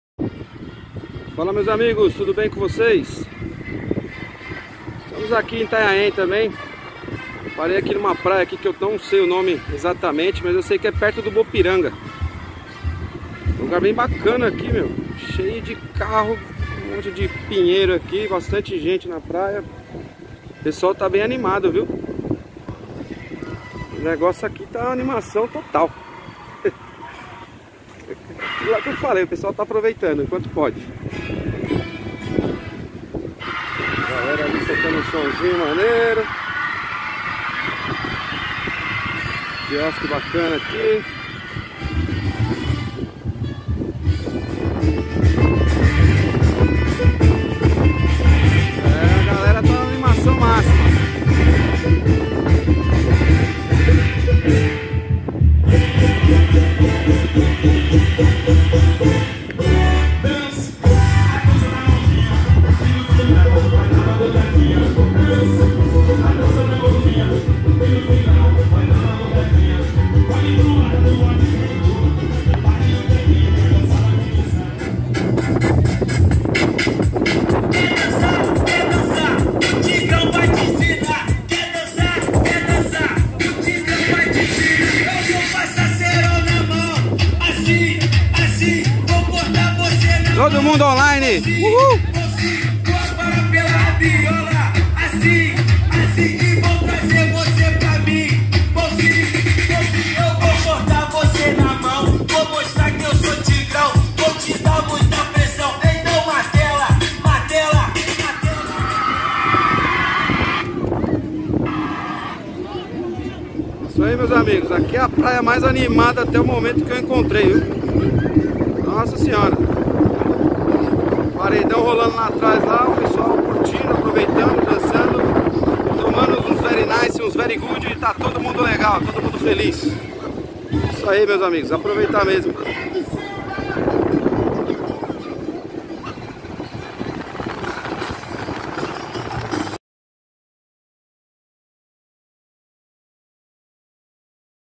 Final de Ano em Itanhaém 2020/2021- Paredão tocando e a galera dançando 😳😬 Litoral Sul de S.P
A Galera estava muito animada. Fiquei impressionado.
Cê tá doído. ...🤪🤪